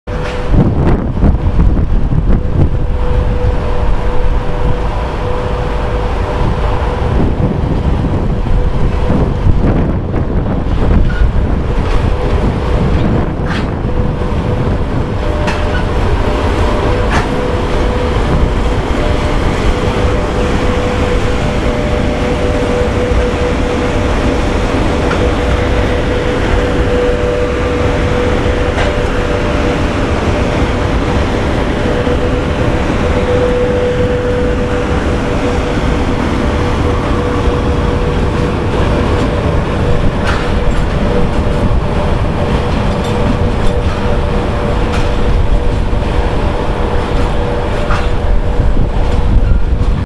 Электровоз ВЛ80С-2666/2665
Электровоз ВЛ80С-2666/2665 совершает манёвры по станции Батайск, г. Батайск, Ростовская область, Северо-Кавказская железная дорога, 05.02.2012 г.